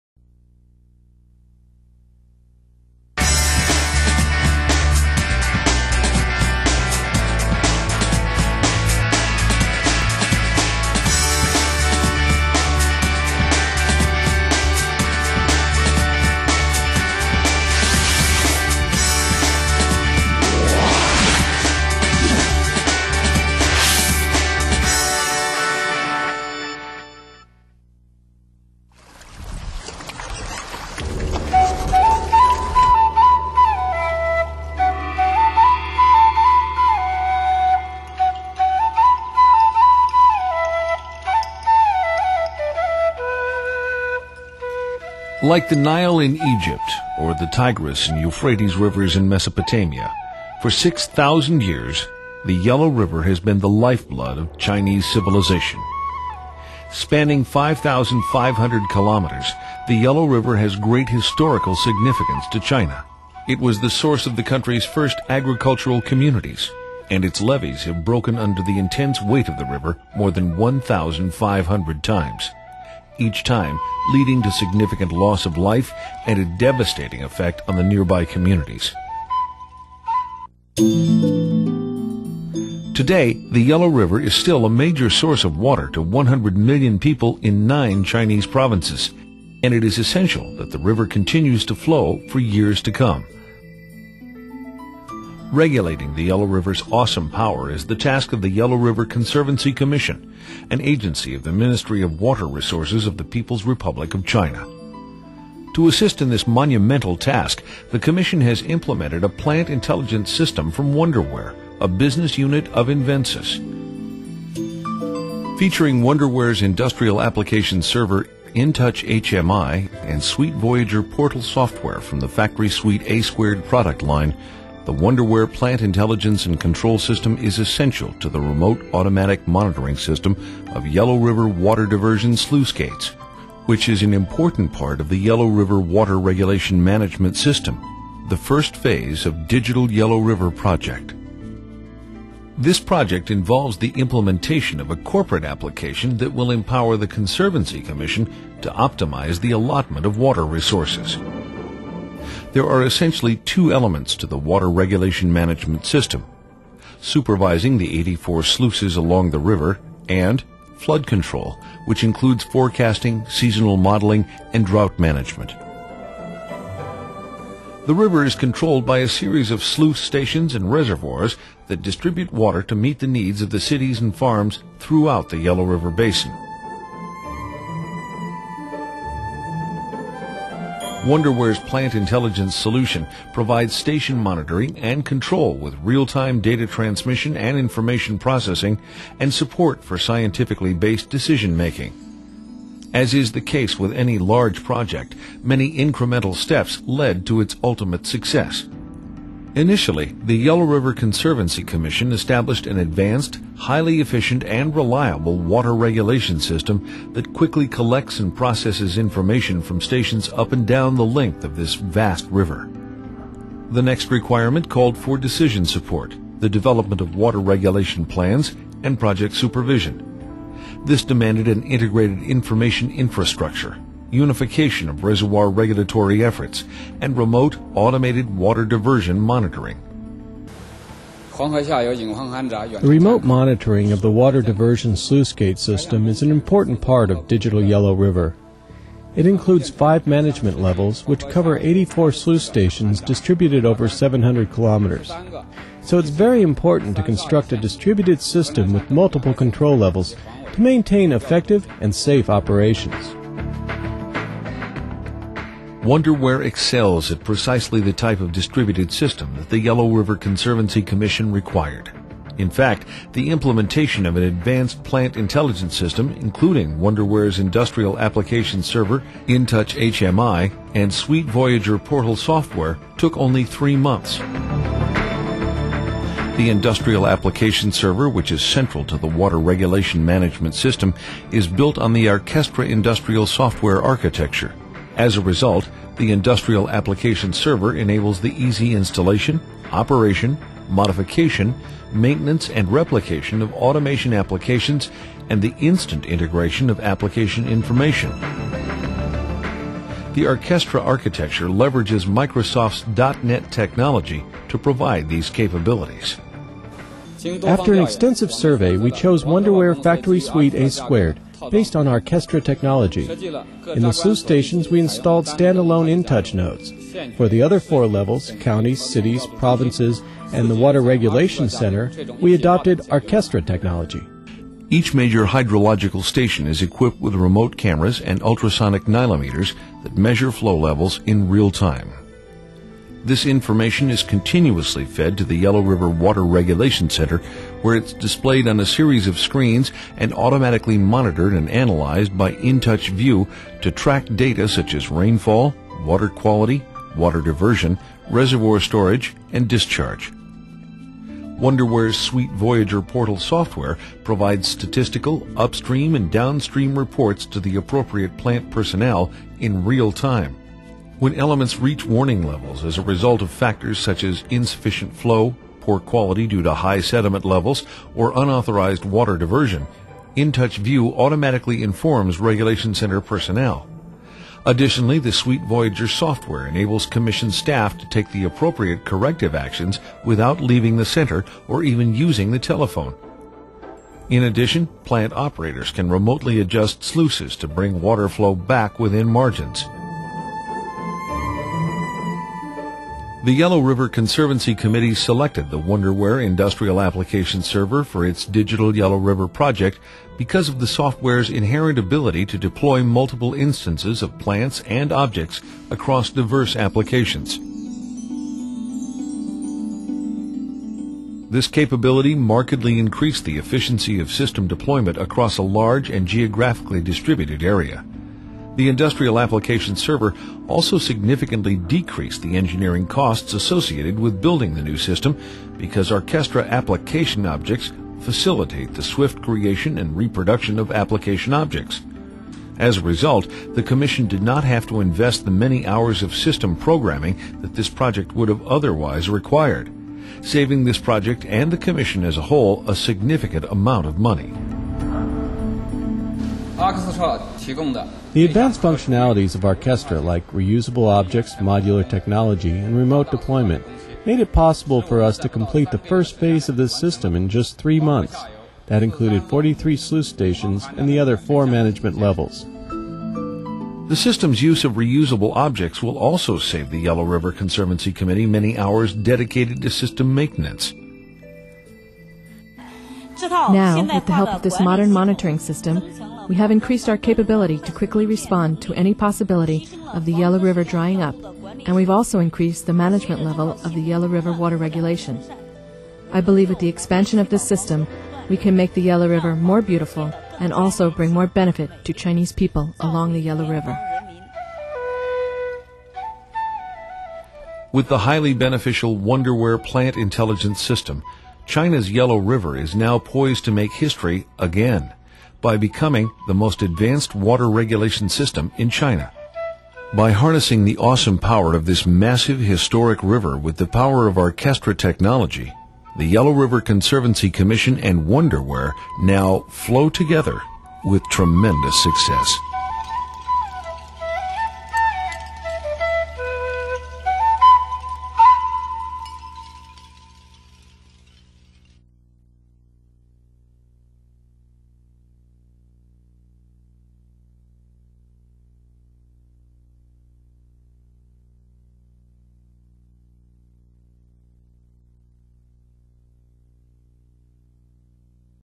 Success Story: Yellow River Conservancy Commission
To regulate the Yellow River, which spans nine Chinese provinces, the Yellow River Conservancy Commission selected a System Platform solution from Wonderware.